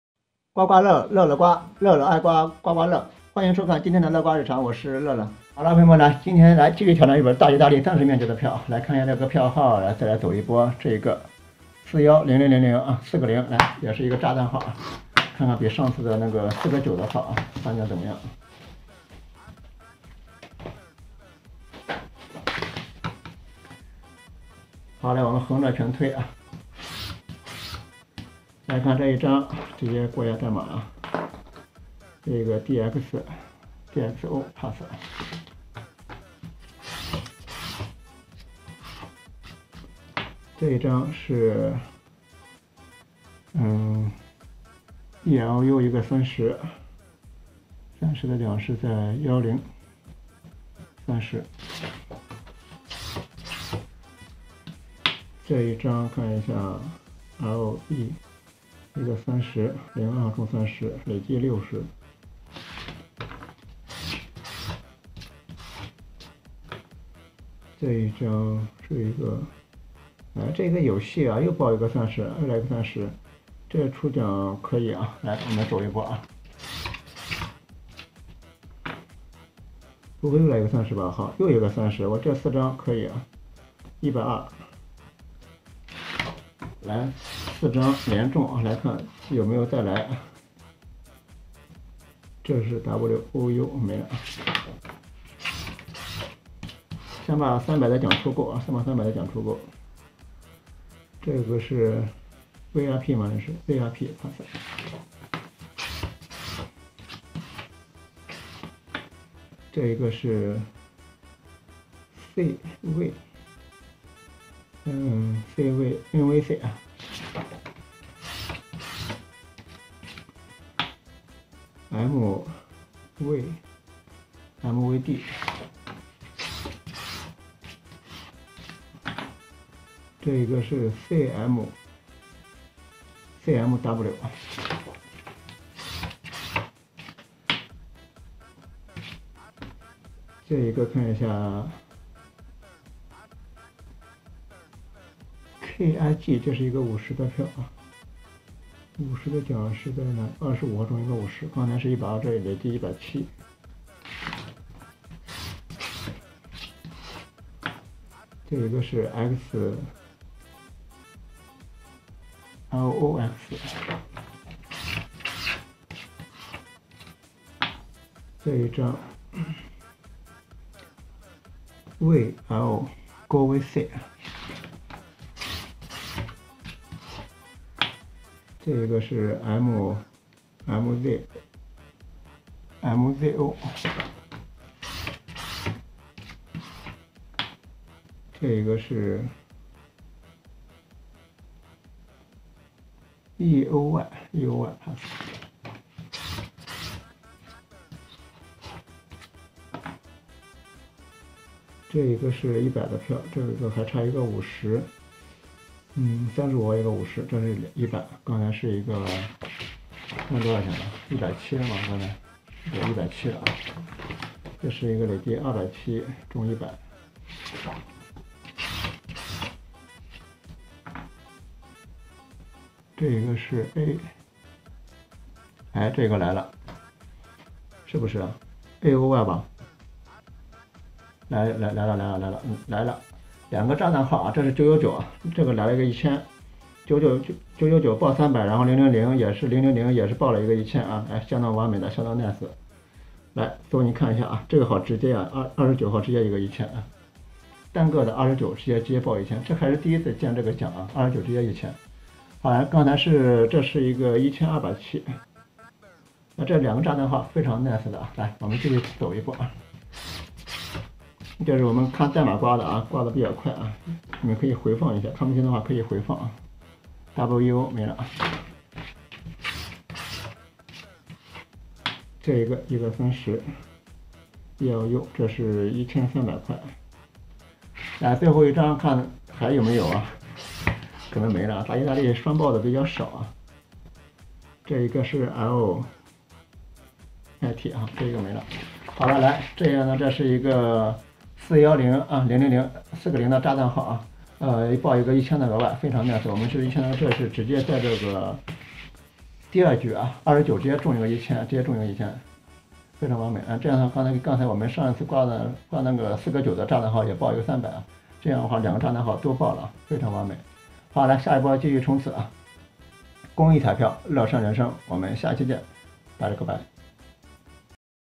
乐刮VLOG：代码速刮，今日0000 PK昨日9999，连号炸弹号够炸炸炸！
票友们想看什么票可以在下方评论区留言哈。
准确评论视频里出现的三个幸运数字。
有机会获赠奖票一张哦。